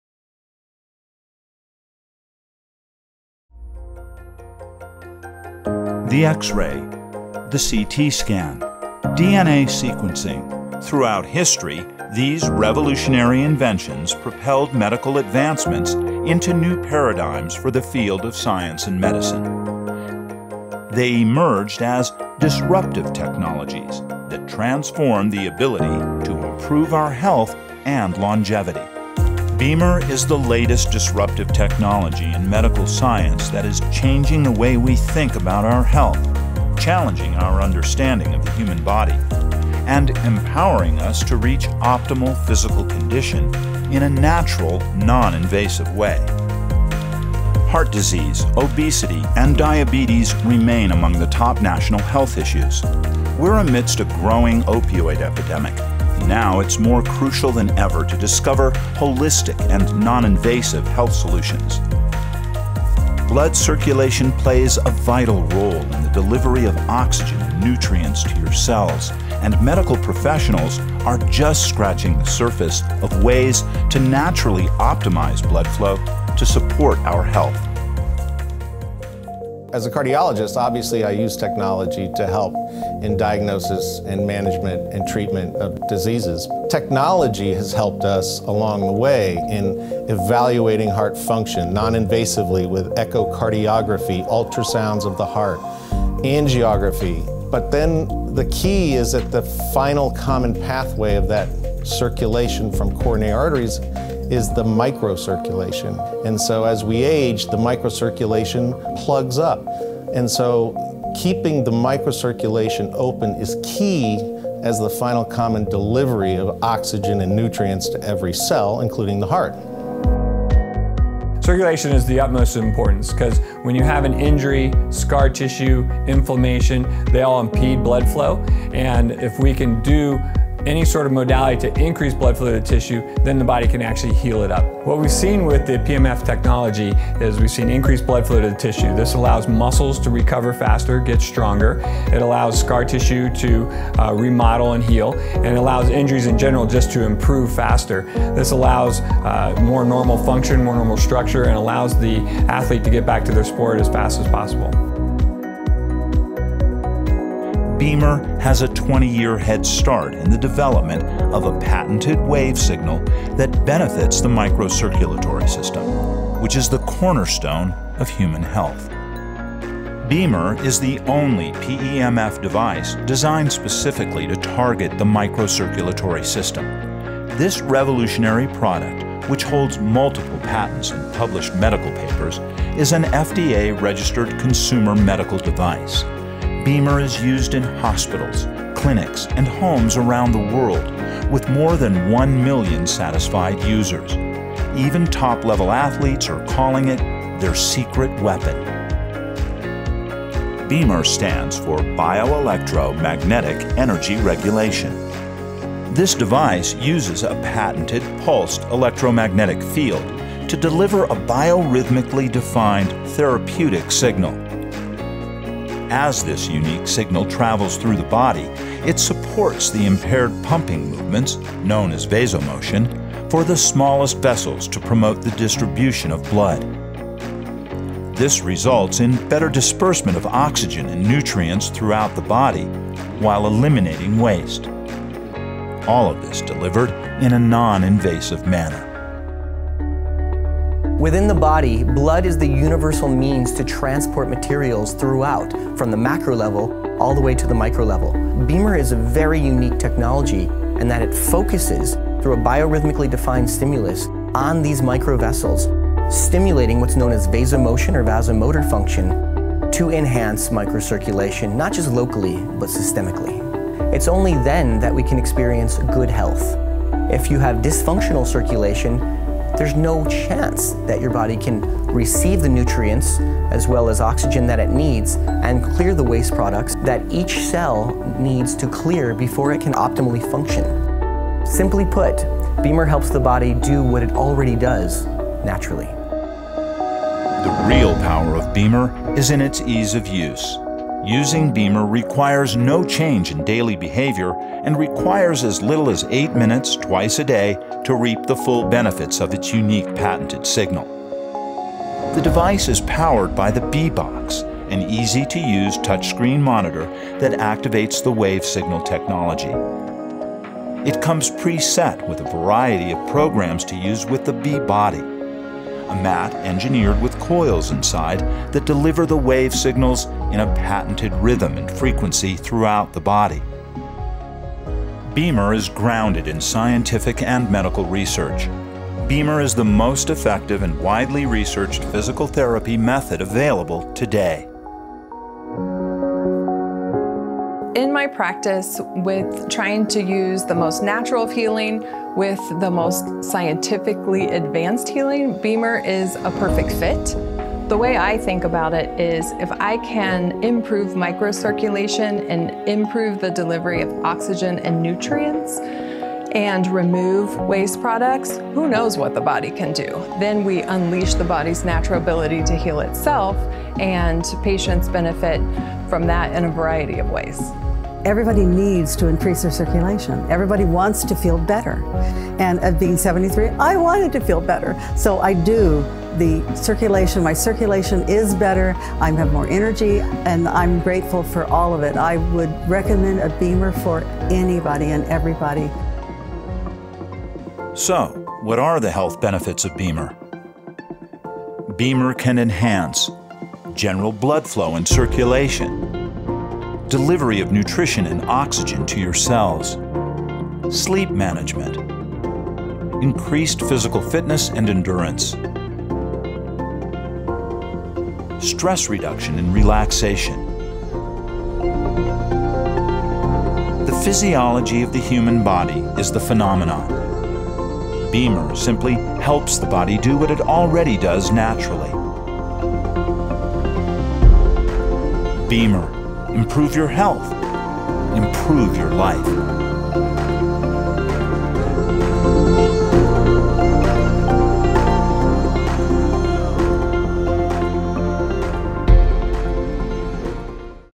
What BEMER is and can do - the eight-minute story about microcirculation | EN
In this video, you can see how BEMER works and how vascular therapy can improve blood circulation. Doctors and experts explain the interrelationships in the human body and show in detail

- how the small and smallest blood vessels in our body work
- how microcirculation deteriorates in the course of our lives and what consequences this can have
- how BEMER physical vascular therapy can help after injuries or in prevention
- how the restricted pumping movements of the small and smallest vessels are improved
- how self-healing powers are activated and energy is increased
- what health benefits you can expect